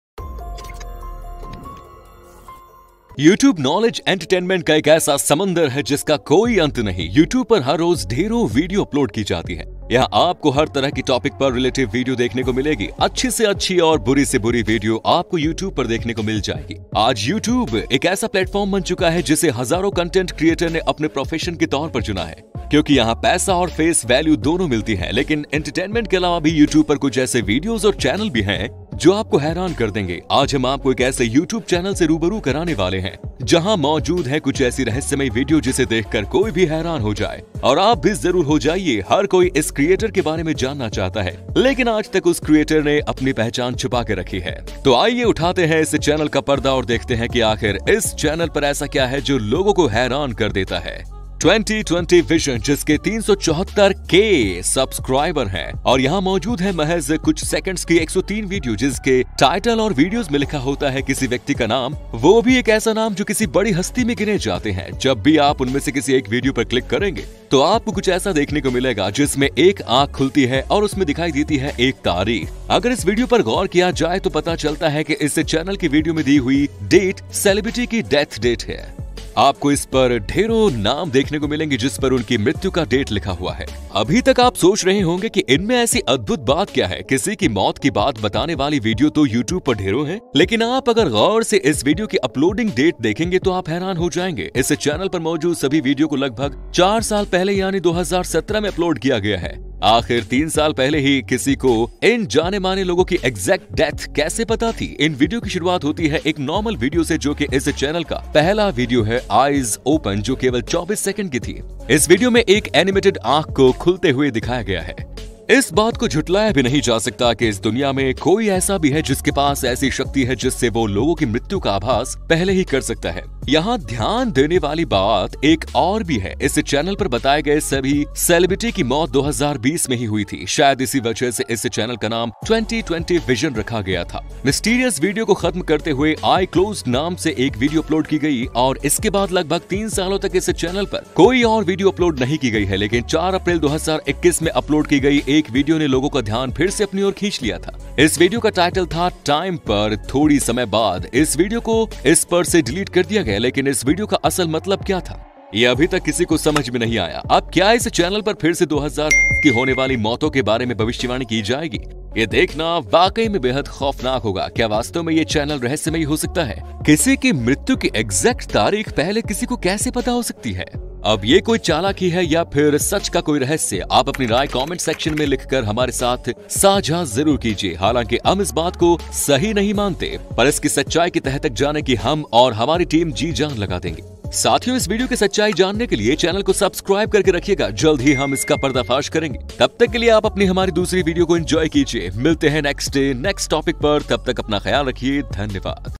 इस चैनल की सच्चाई जानकर आपके होश उड़ जाएंगे | Reality of Vision 2020 Youtube Channel in Hindi
Youtube  नालेज एंटरटेनमेंट का एक ऐसा समुंदर हैं, जिसका कोई अंत नहीं, youtube पर हर रोज ढेरों वीडियो अपलोड की जाती हैं,  यहा आप को हर तरह की टॉपिक पर रिलेटिव वीडियो देखने को मिलेगी, अच्छे से अच्छी और बुरे से बुरी वीडियो आपको youtube पर देखने को मिल जाएगी, आज youtube एक ऐसा प्लेटफार्म बन चुका है जिसे हजारों कंटेंट क्रिएटर ने अपने प्रोफेशन के तौर पर चुना है, क्योंकि यहां पैसा और फेस वैल्यू दोनों मिलता है, लेकिन इंटरटेनमेंट के अलावा भी youtube पर कुछ ऐसे वीडियोस और चैनल भी है जो आपको हैरान कर देंगे, आज हम आपको एक ऐसे youtube चैनल से रूबरू कराने वाले हैं, जहां मौजूद है कुछ ऐसी रहस्यमय वीडियो जिसे देखकर कोई भी हैरान हो जाए, और आप भी जरूर हो जाएंगे, हर कोई इस क्रिएटर के बारे में जानना चाहता है, लेकिन आज तक उस क्रिएटर ने अपनी पहचान छुपा रखा है,

Youtube is such a sea of knowledge entertainment, which has no end, many videos are uploaded on youtube everyday, here you will get to see relative videos on all kinds of topics, from best to best and from worst to worst videos on youtube. You will get to see, today youtube has become a platform that has been chosen by thousands of content creators as their profession, because both money and face value are available here, but apart from entertainment, there are also some such videos and channels on youtube. Which will surprise you, today we are going to introduce you to such a youtube channel, where there are some such mysterious videos that anyone will be surprised to see, and you will surely be, everyone wants to know about this creator. But till date that creator has kept his identity hidden.